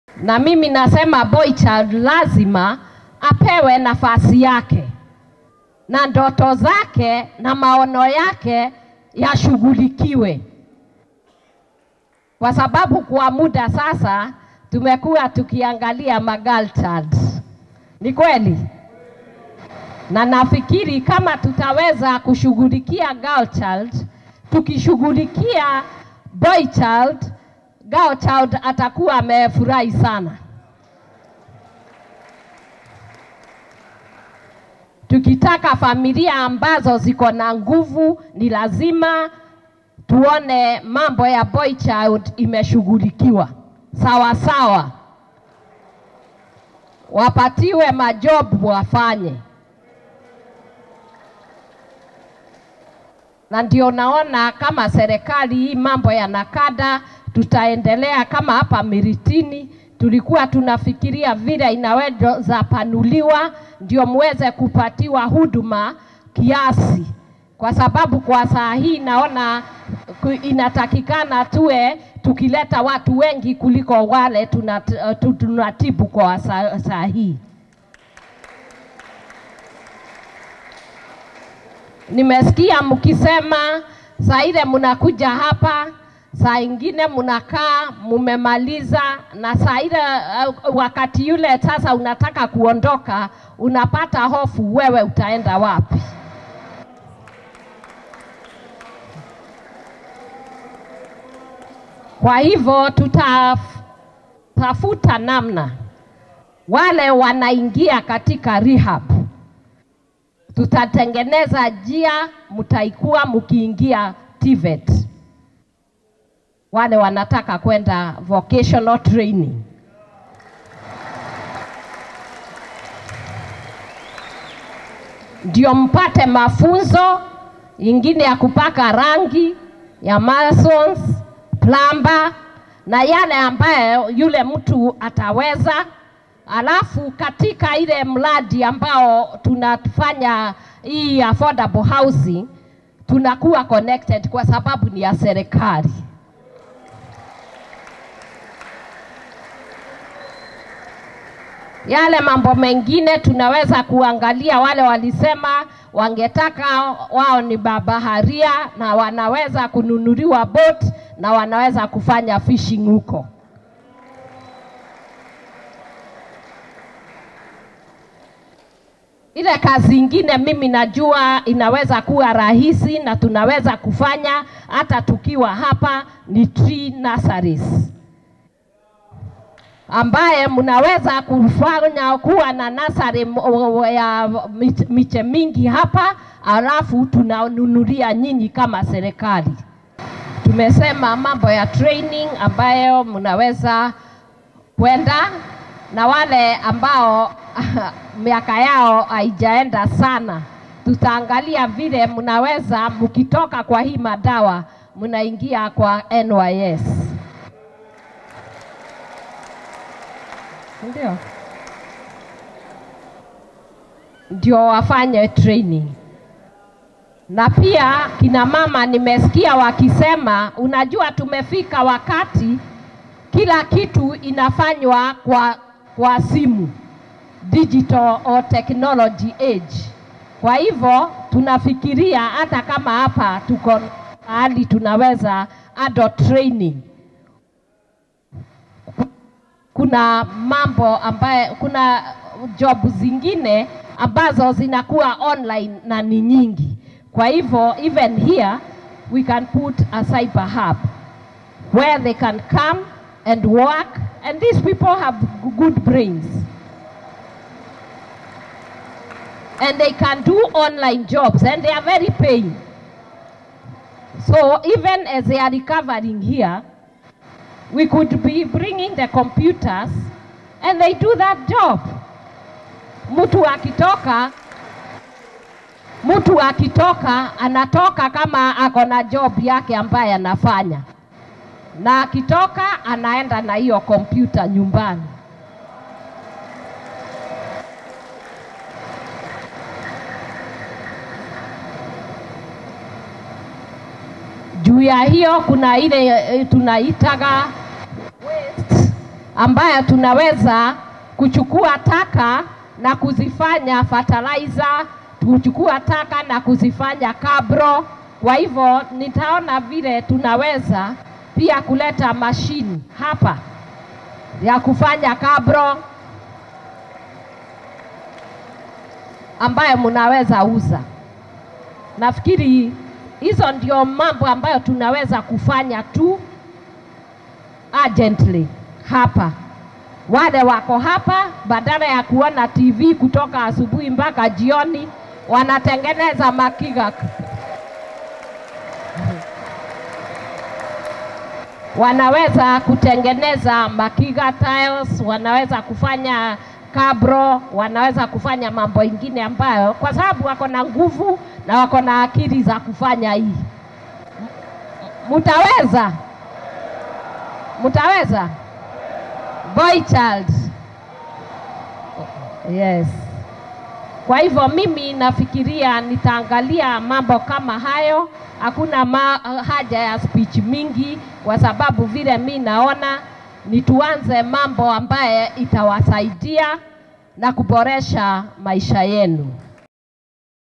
MIRITINI TREATMENT AND REHABILITATION CENTRE VISIT
Glad to have met hundreds of recovering addicts at the Miritini Treatment and Rehabilitation Centre in Mombasa County. I urged them to remain committed in the path of recovery, even as the government continues to work around the clock for an end-to-end rehabilitation process that includes skilling, retooling, TVETs, jobs & entrepreneurship. The youth are the future, and we must restore and empower them.